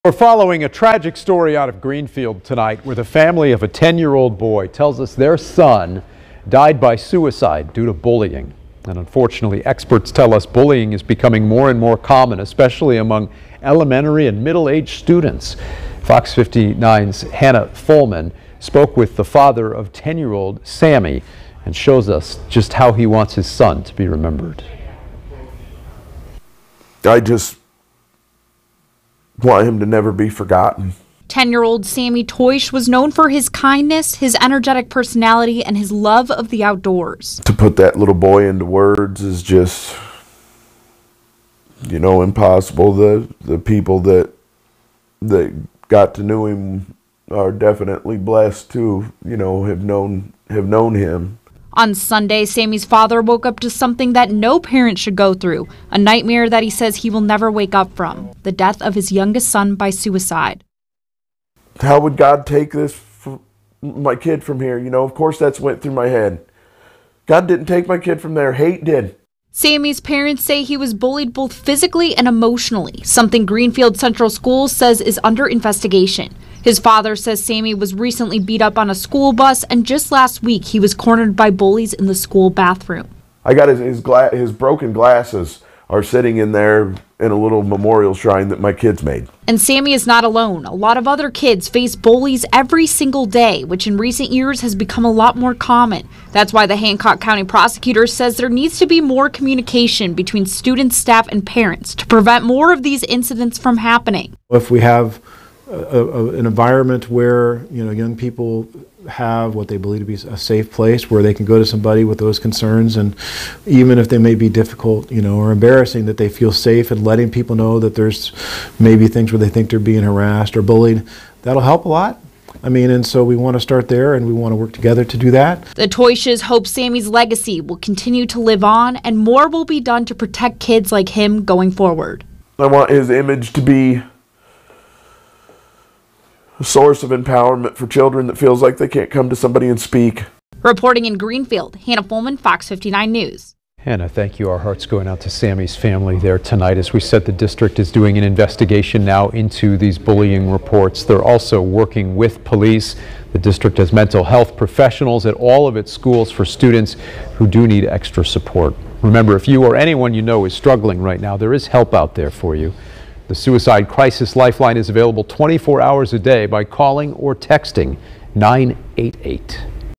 10-year-old Greenfield boy dies by suicide amidst bullying concerns
10-year-old Greenfield boy dies by suicide amidst bullying concerns